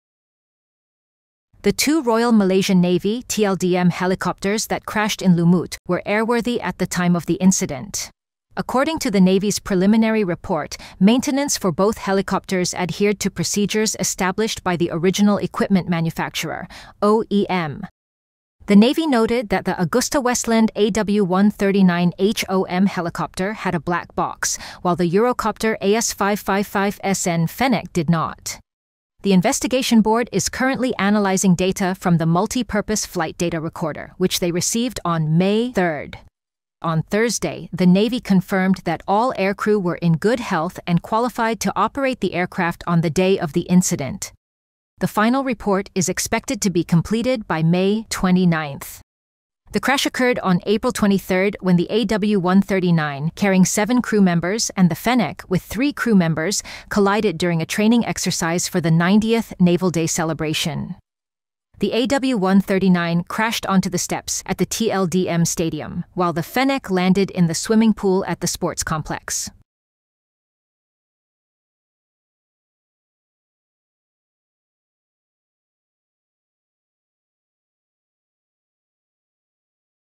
Copter tragedy: Helicopters involved were airworthy, all crew fit at time of incident
The two Royal Malaysian Navy (TLDM) helicopters that crashed in Lumut, Perak were airworthy at the time of the incident, according to the preliminary report on the tragedy.

In a statement on Thursday (May 9), the Navy also said that all air crewmen were in fine health to handle the aircraft and fit to do so based on their qualifications on the day of the incident.